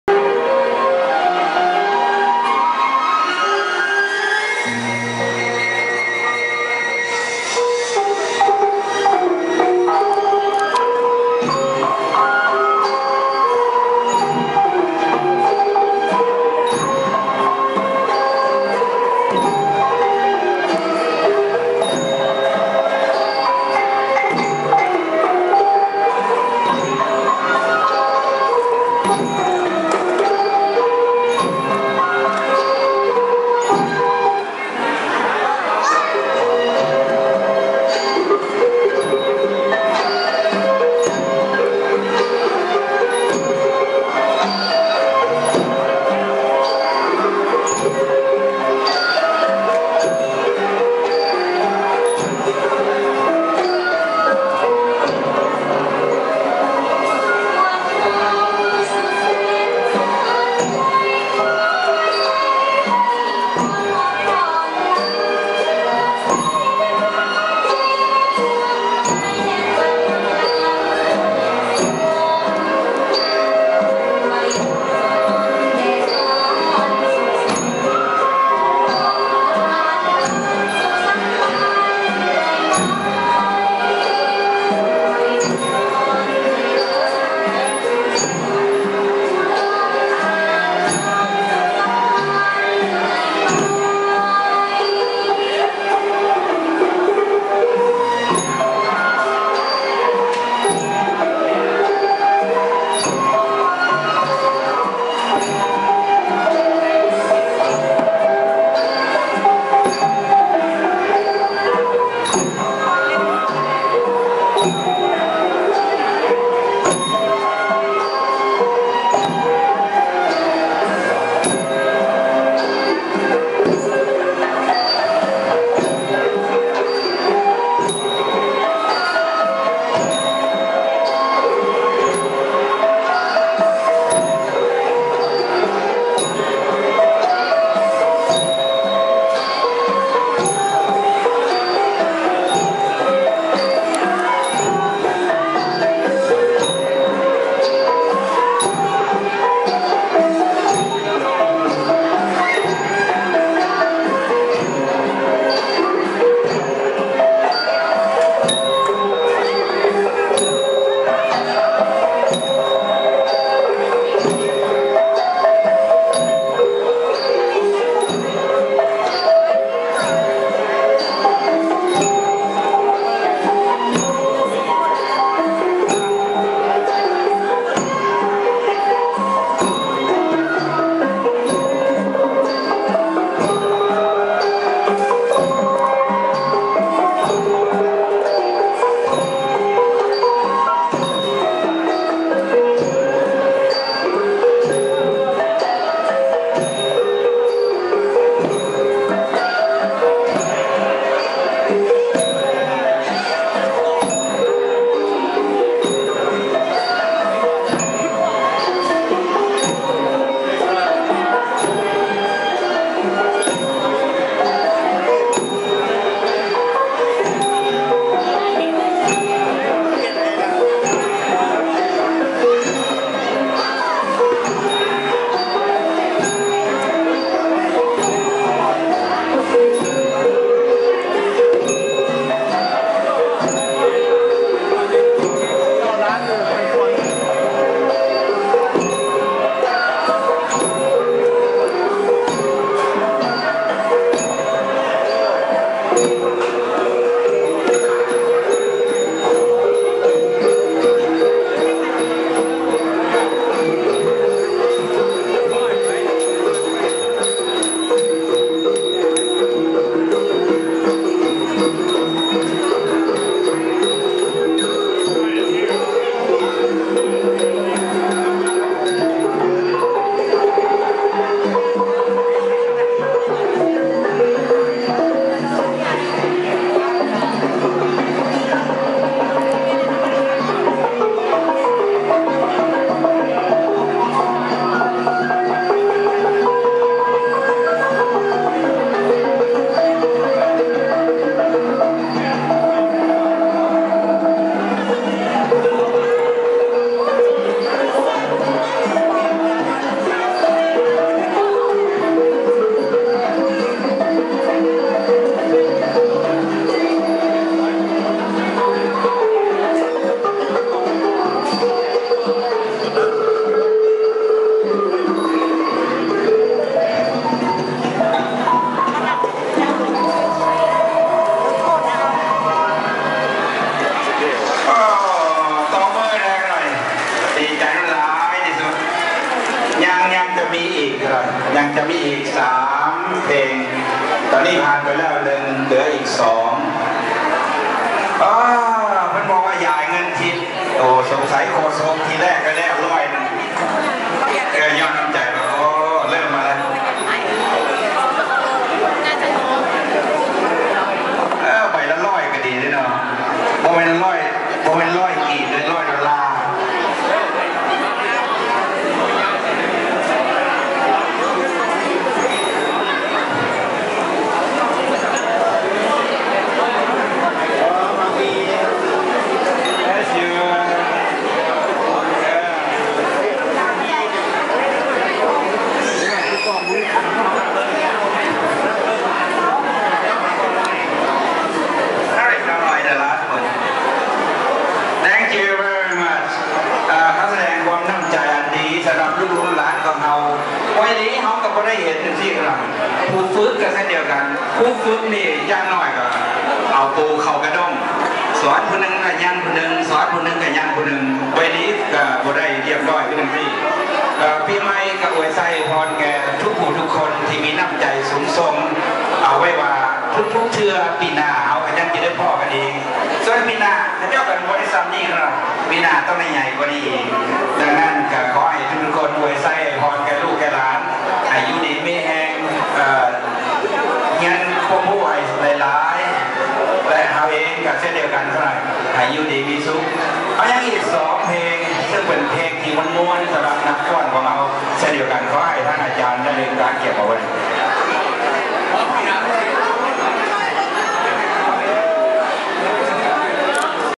Lao Folk Dancers at WatBuddhaSamakidham 2015
Lao New Years Party at the WatBuddha Samakidham
Apri 11 2015 ..Lao Folk Dancers at WatBuddhaSamakidham 2015